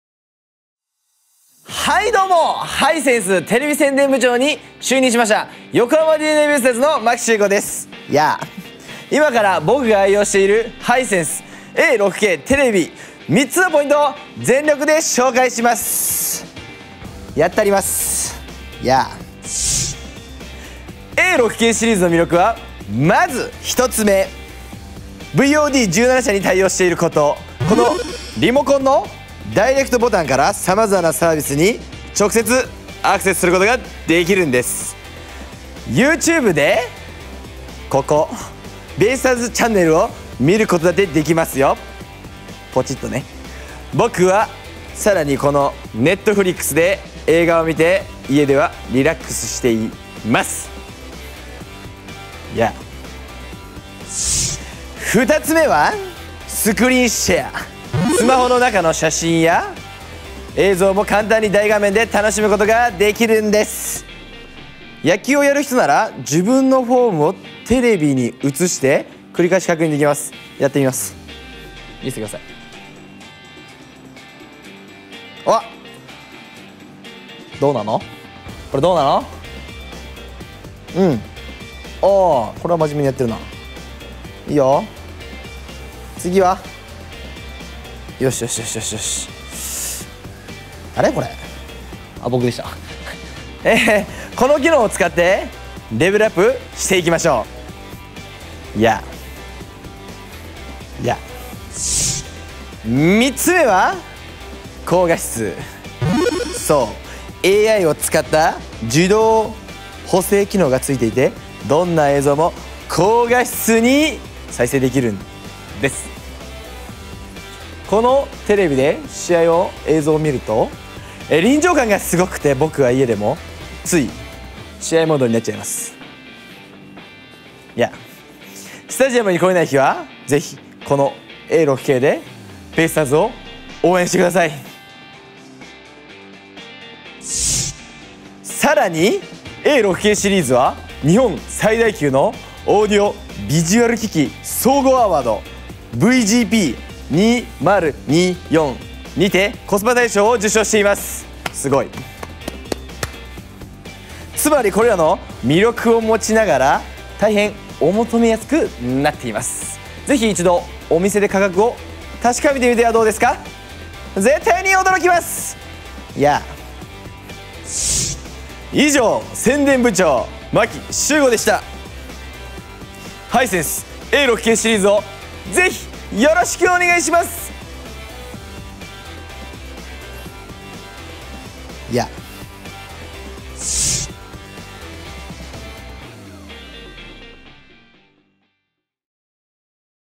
横浜DeNAベイスターズ 牧秀悟選手の全力商品紹介 ハイセンスA6K/E6K
ハイセンス宣伝部長の牧選手にハイセンスの新商品テレビA6K/E6Kシリーズの全力商品紹介を、牧選手オリジナルにアレンジされた内容でお届けします。

【販売サイト一覧】
・Amazon

【商品ページ】
・E6Kシリーズ

[ご提供]
朝日木材加工株式会社「リビングボード SP-STN2080」
ハイセンスジャパン株式会社
広報： 家倉宏太郎